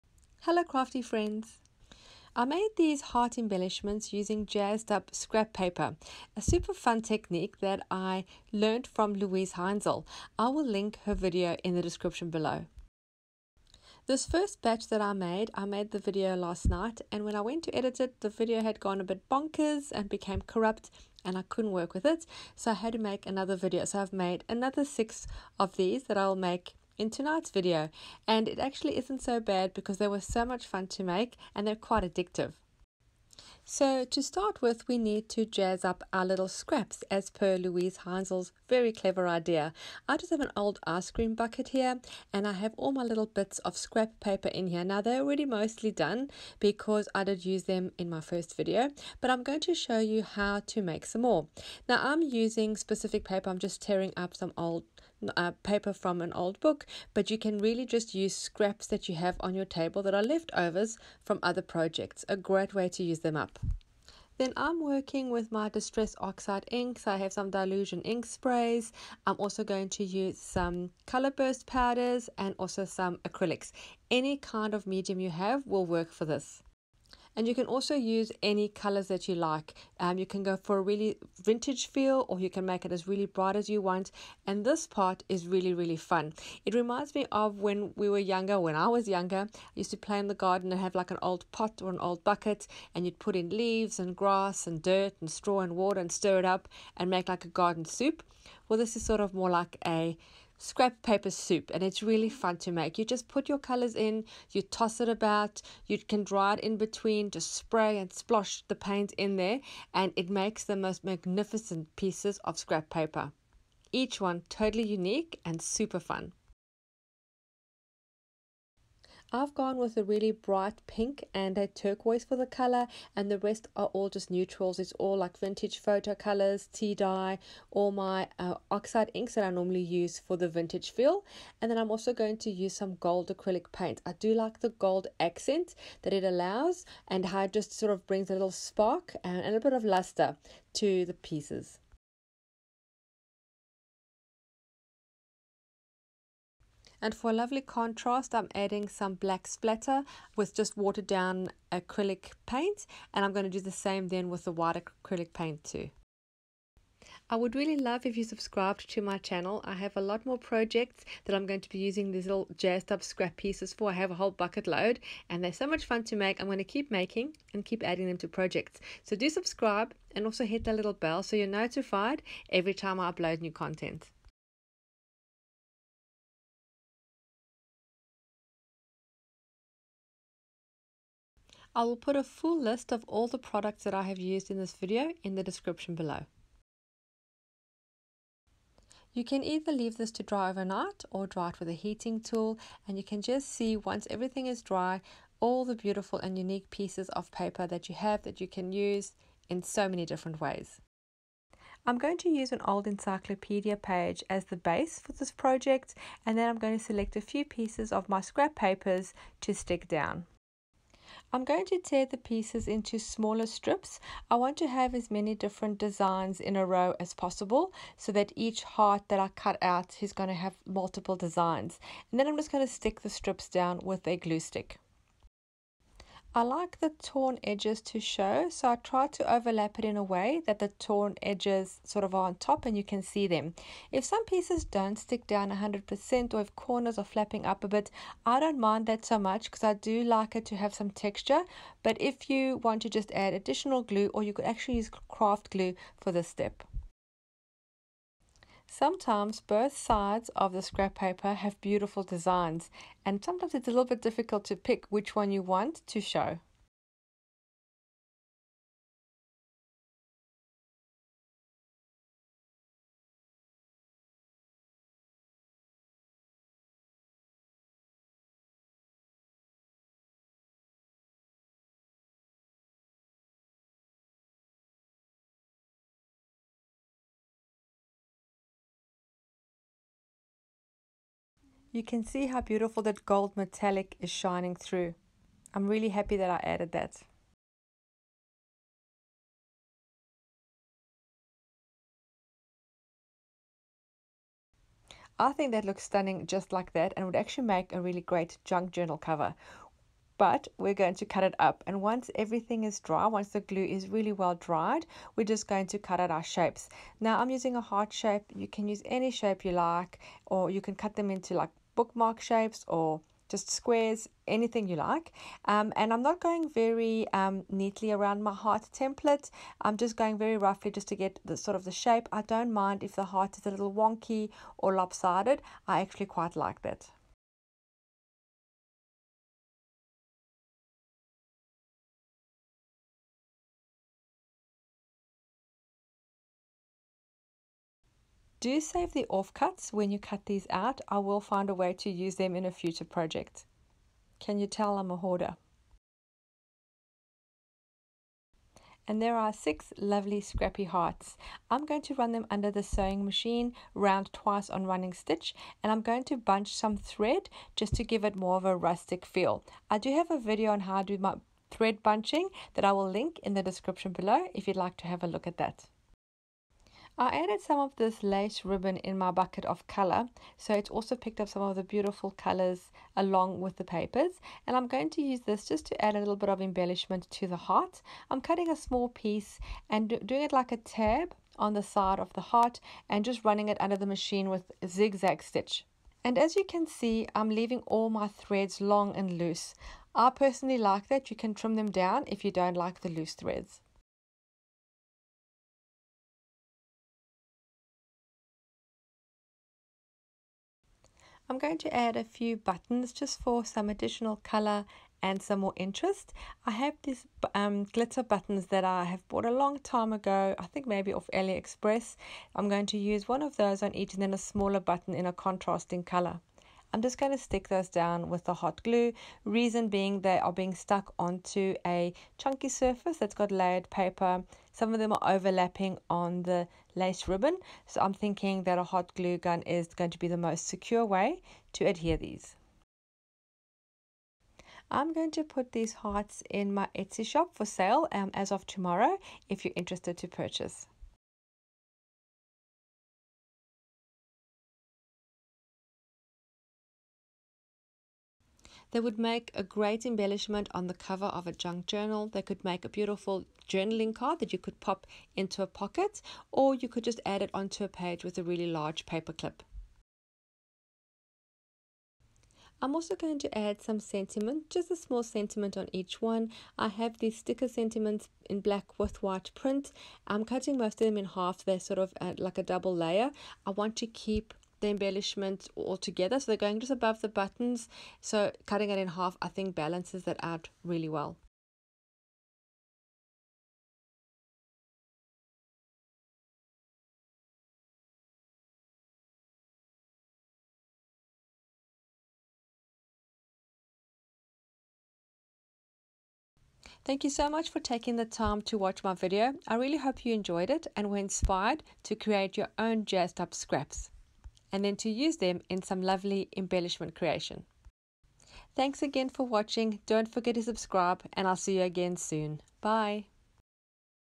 Create Heart Embellishments using scraps 🦋 ShanoukiArt 🦋🧿
A really fun way to jazz up your scraps inspired by Luise Heinzl and then create heart embellishments.

Products I  used:
Tim Holtz Distress Oxide Spray: Vintage Photo, Tea Dye, Brushed Corduroy, Antique Linen
Dylusions Ink Spray: Desert Sand, White Linen
Ken Oliver Color Burst Powders: Turquoise, Fuchsi
Craft Acrylic Paint: White, Black, Gold (from Big W Australia)
Lace Ribbon
Buttons

🛒Printables from Digital Collage Club

I would love to see what you create, please use hashtag shanoukiart so that I can see your creations.

Happy creating!